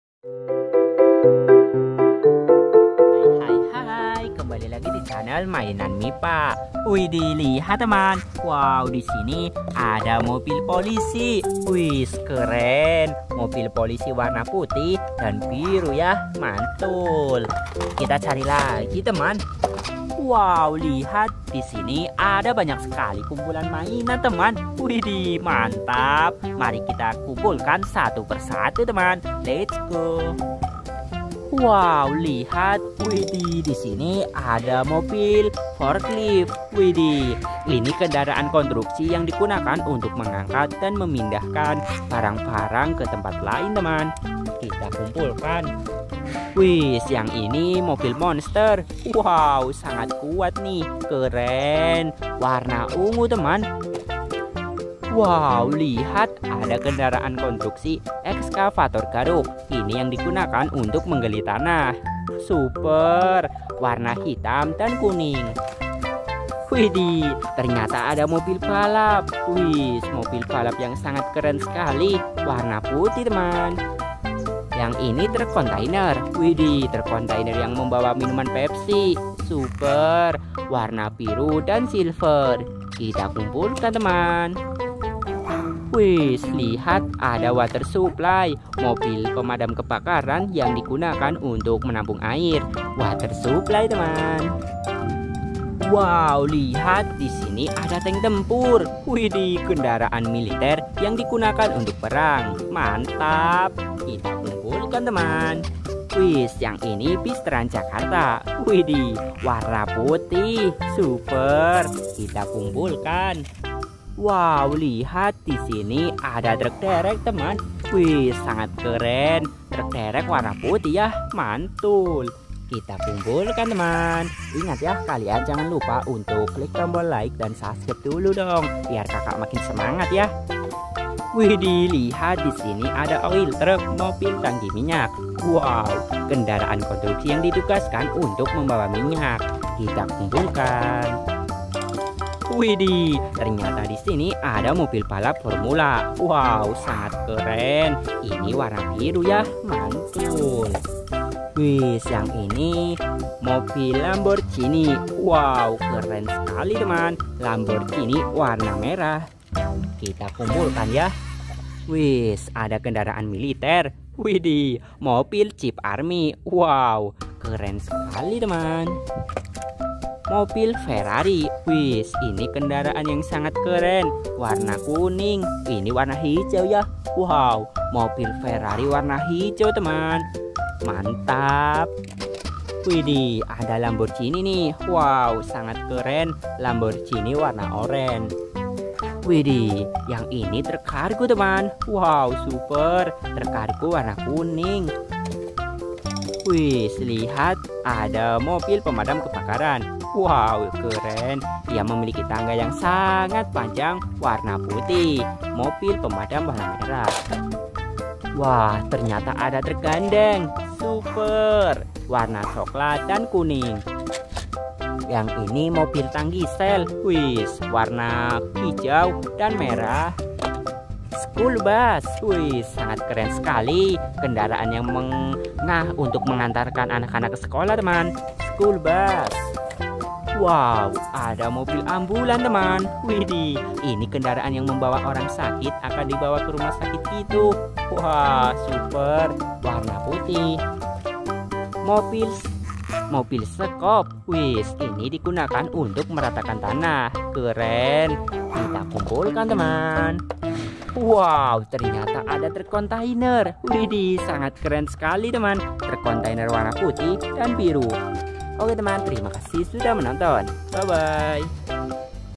AJIB BENER NIH !! MAINAN SERBA ADA INI COCOK UNTUK ANAK ANAK, MOBIL DEREK SAMPAI KE MOBIL DAMKAR
mainan anak,
review mainan,